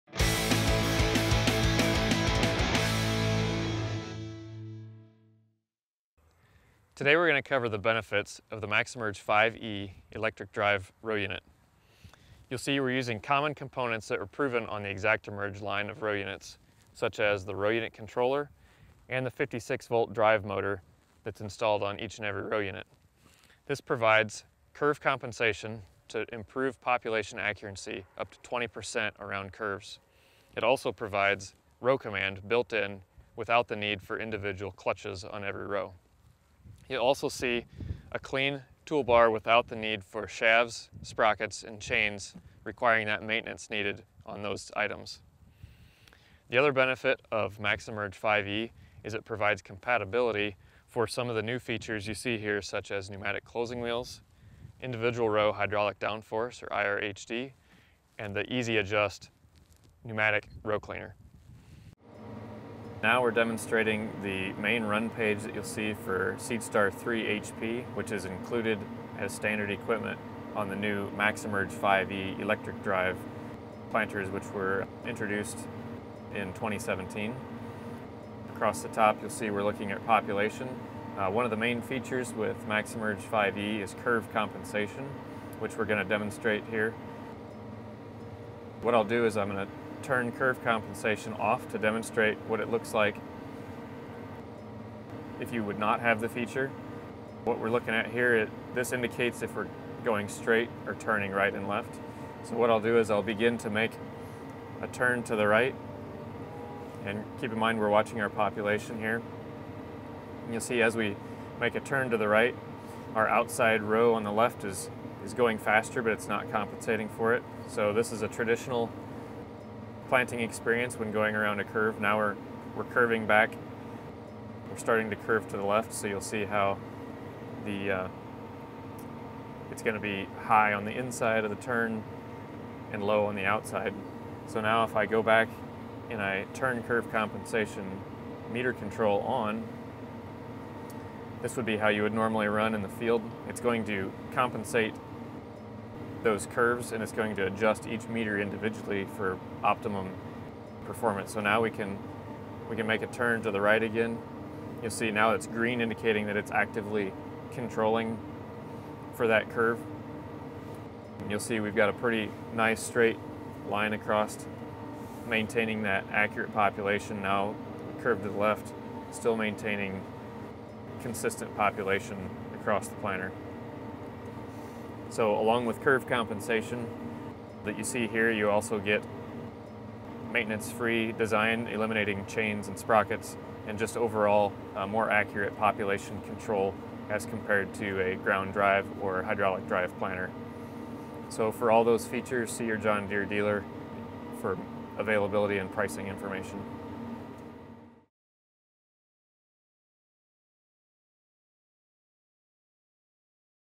MaxEmerge(TM) 5e - Electric Drive Row Unit
Understand the capabilities of the John Deere MaxEmerge(TM) 5e row unit. This electric drive row unit enables curve compensation and can be ordered with Individual Row Hydraulic Downforce, Pneumatic Closing Wheels and Easy Adjust Row Cleaners.